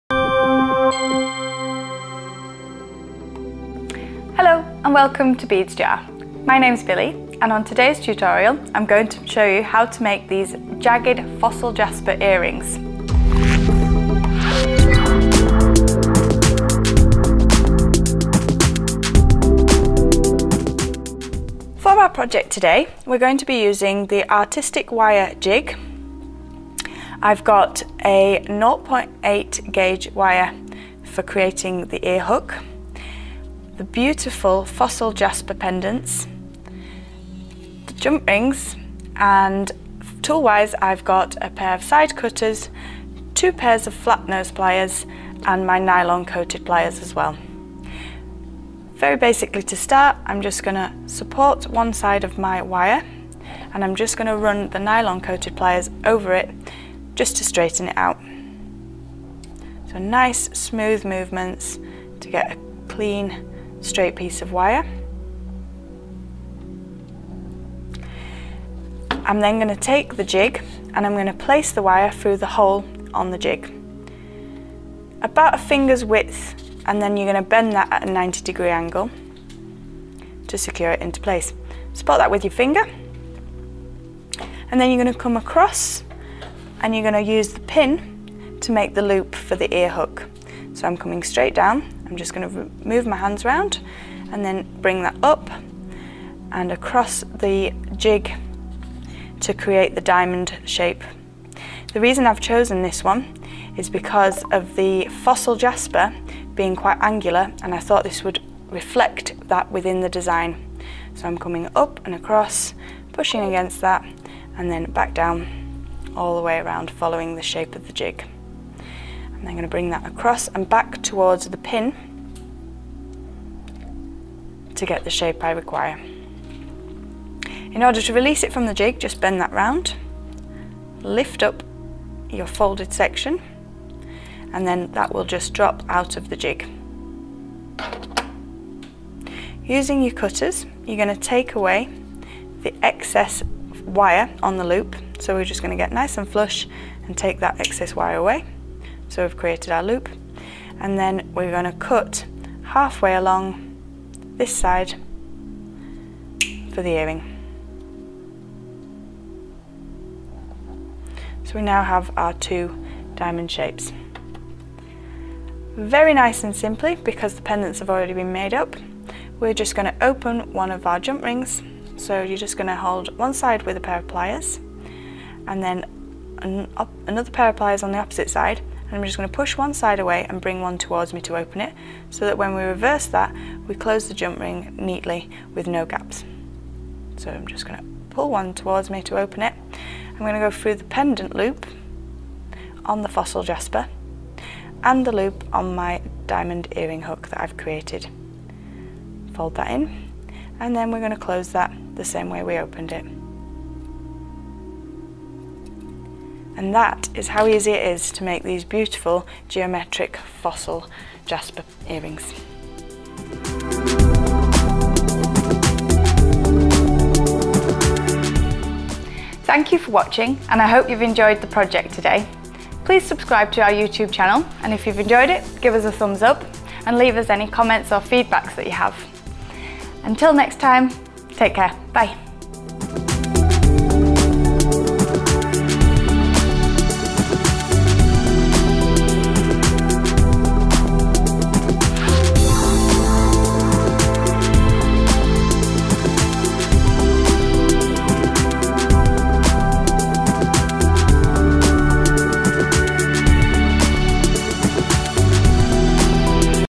How to make these Jagged Fossil Jasper earrings
In this quick tutorial you can see how to use the Artistic wire jig to make your very own diamond ear hooks and connect them to the beautiful Fossil Jasper pendants.